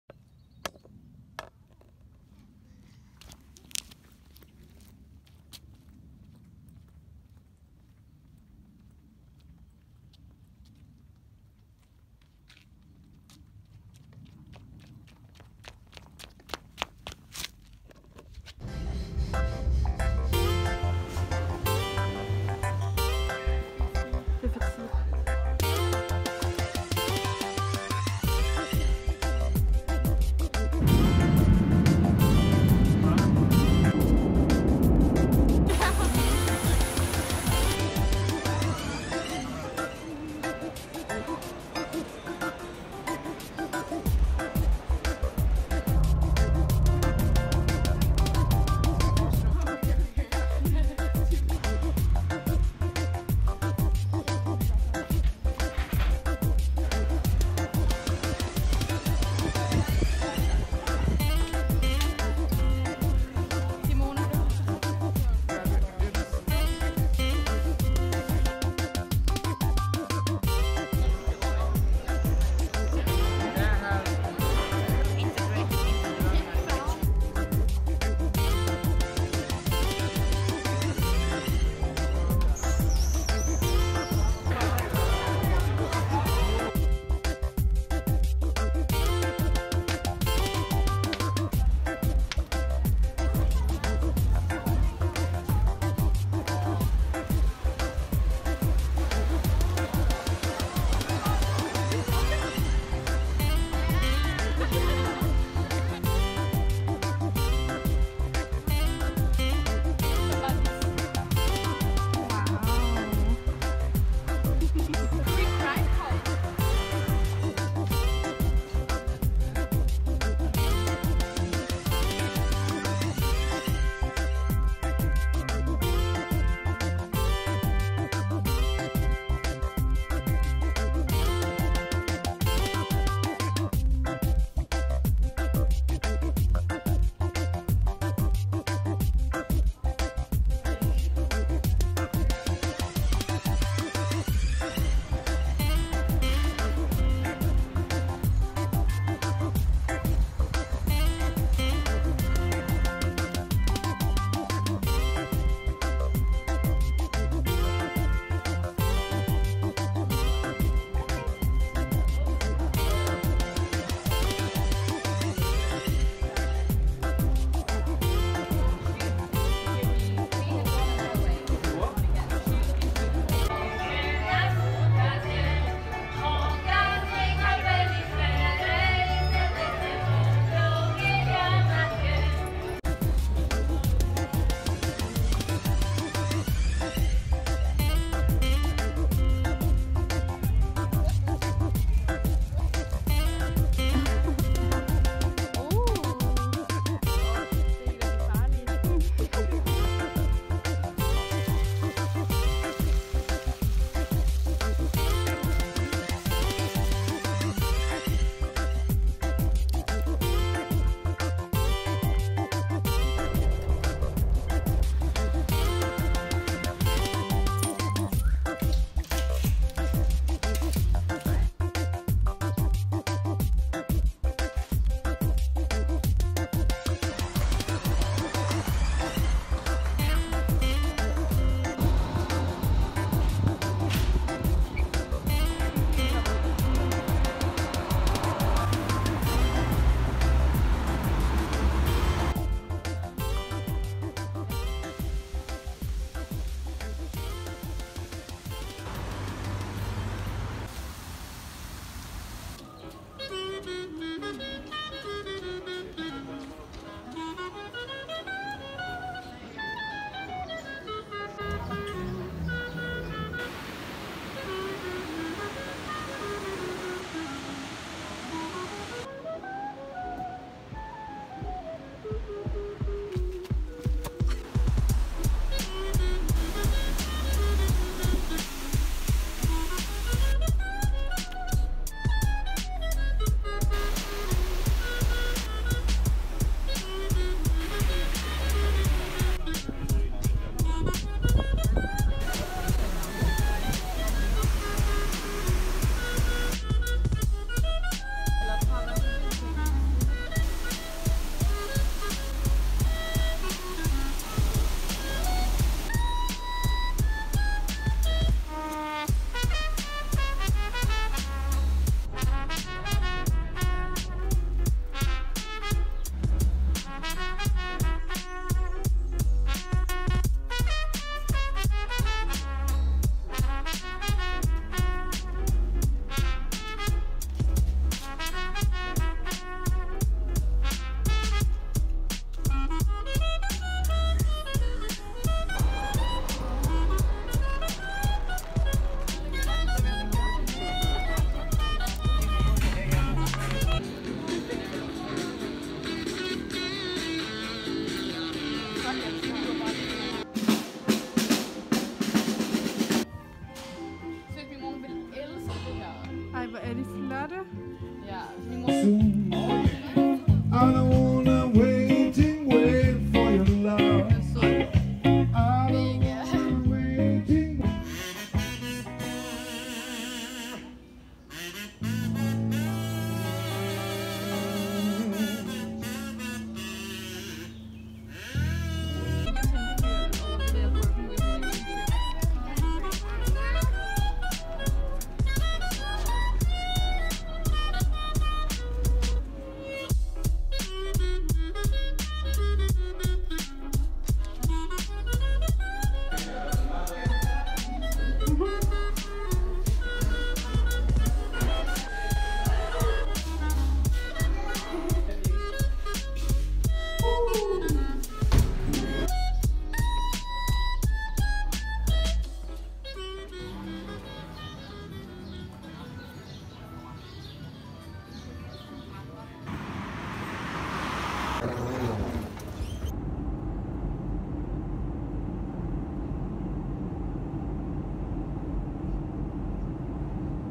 Kroatien | Split + Zagreb
En videodagbog fra Kroatien 2019 :))